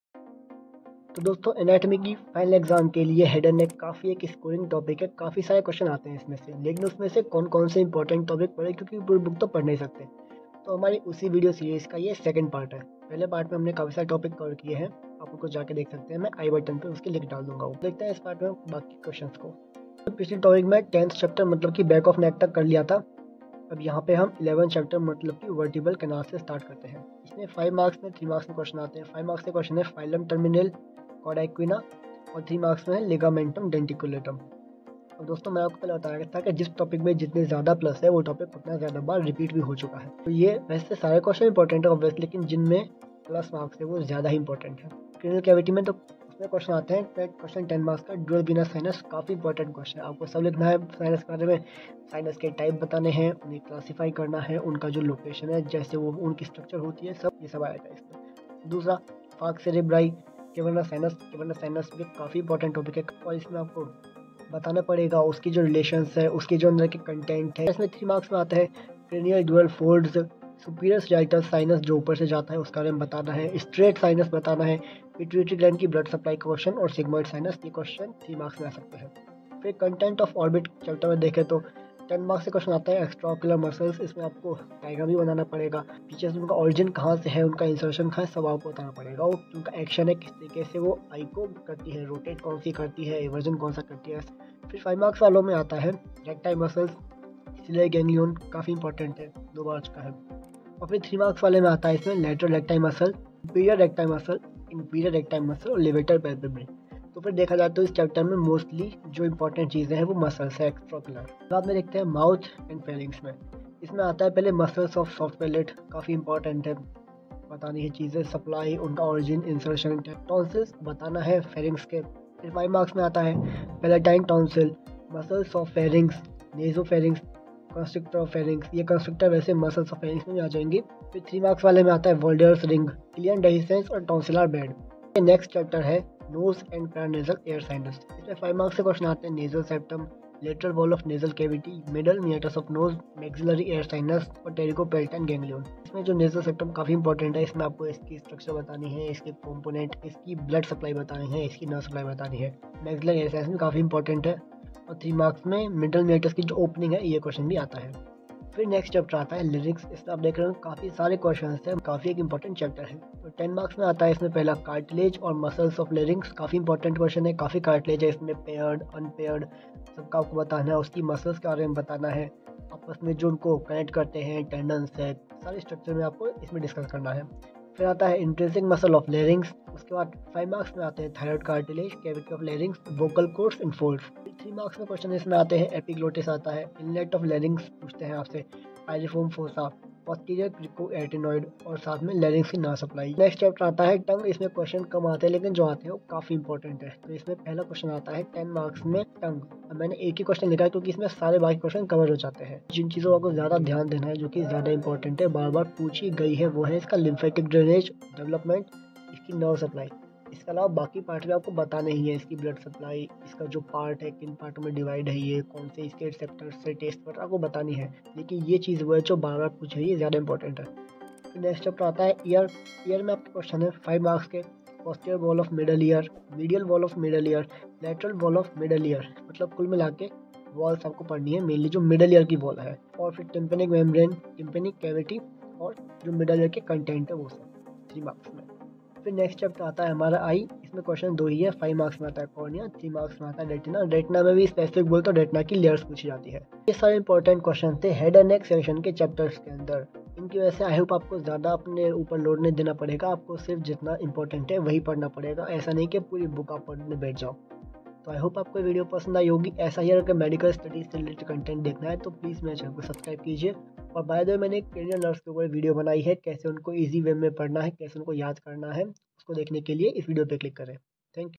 HEAD AND NECK important topics |part 2|FINAL EXAMS
This video teach the important topics to read in first year anatomy final exams rapidly in less time in head and neck topic,all the must read questions are there

For doubts, gmail-